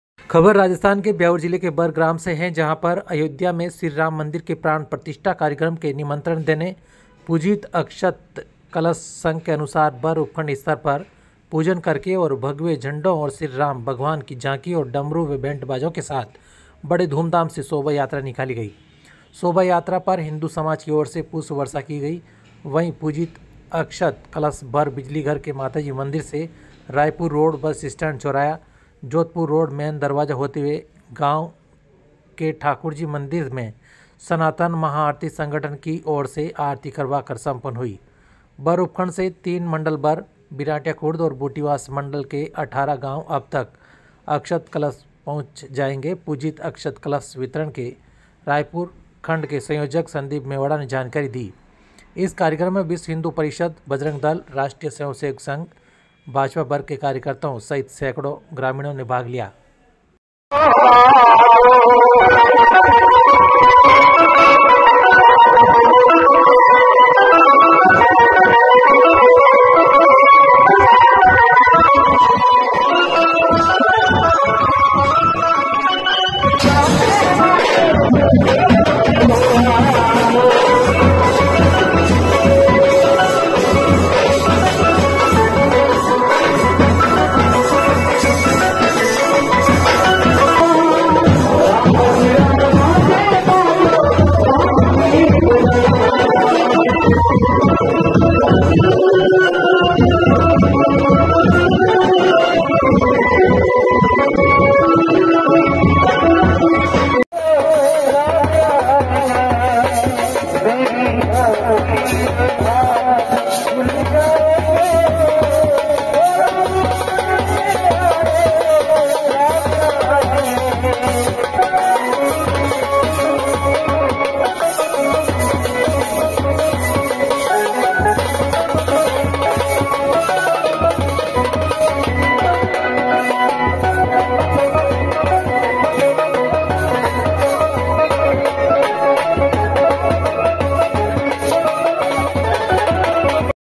भगवे झंडो_श्री राम् भगवान की झांकी और डमरु व बैंड बाजो के साथ बड़े धूमधाम से निकाली गयी शोभायात्रा
अयोध्या मे श्रीराम् मंदिर के प्राण प्रतिष्ठा कार्यक्रम के निमंत्रण देने पुजीत अक्षत कलश, संघ के अनुसार बर उपखंड स्तर पर पूजन करके और भगवे झंडो और श्री राम् भगवान की झांकी ओर डमरु व बेंड बाजो के साथ बड़े धूमधाम से शोभायात्रा निकाली गयी। शोभायात्रा पर् हिन्दु समाज की ओर से पुष्प वर्षा की गई। पुजीत अक्षत कलश बर बिजली घर के माताजी मंदिर से रायपुर रोड़ बस स्टेण्ड चोराया जोधपुर रोड़ मेन दरवाजा से होते हुए। गाँव के ठाकुर जी मंदिर मे सनातन महाआरती संघठन की और से आरती करवा कर सम्पन हुई। बर उपखण्ड से तीन मंडल बर, बिराठिया खुर्द और बुटीवास मंडल के अठारह गांव तक अब् अक्षत कलश पहुँच जाएंगे। पुजीत अक्षत कलश वितरण के रायपुर खंड के संयोजक संदीप मेवाड़ा ने जानकारी दी। इस कार्यक्रम मे विश्व हिन्दु परिषद बजरंग दल , राष्ट्रीय स्वयंसेवक संघ, भाजपा बर् के कार्यकर्ताओ के सहित सेकड़ो ग्रामीणों ने भाग लिया।